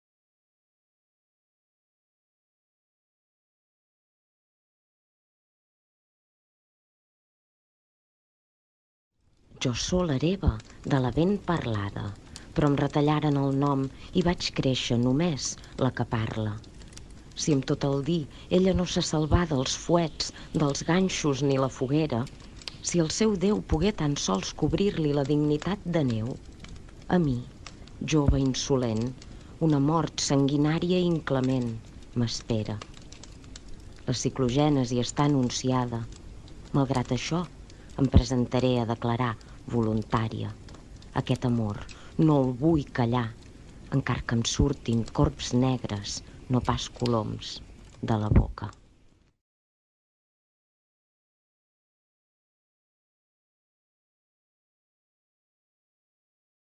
Laia Malo
Laia Malo és una de les poetes de l’antologia «Massa mare», a cura de Maria Cabrera i amb la proposta visual de Raquel Tomàs. Per a l’ocasió, ha escollit un metrònom com a objecte per dialogar visualment amb els versos.